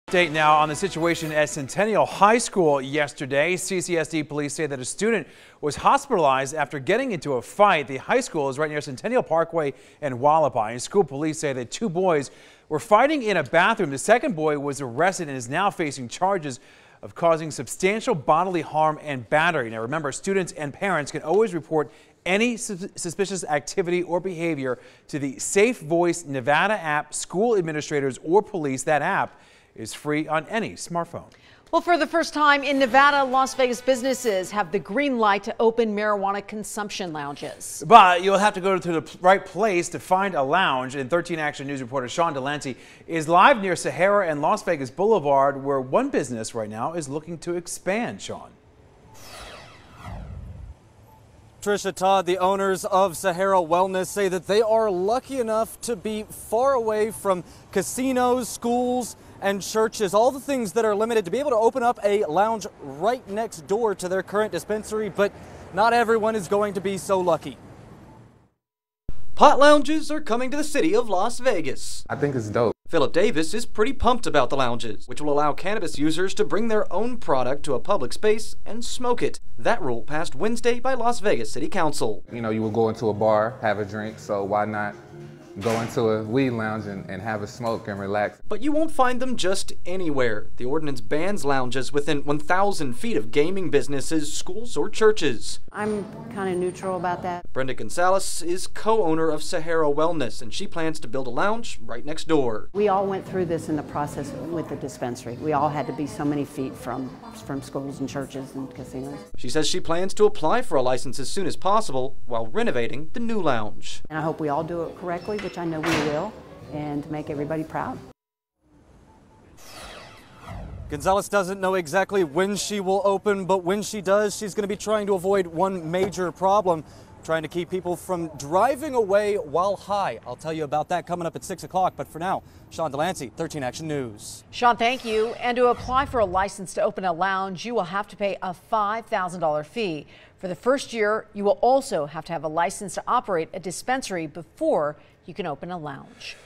Pot lounge legalization comes with restrictions for Vegas business owners
Las Vegas Marijuana Lounges Come With Restrictions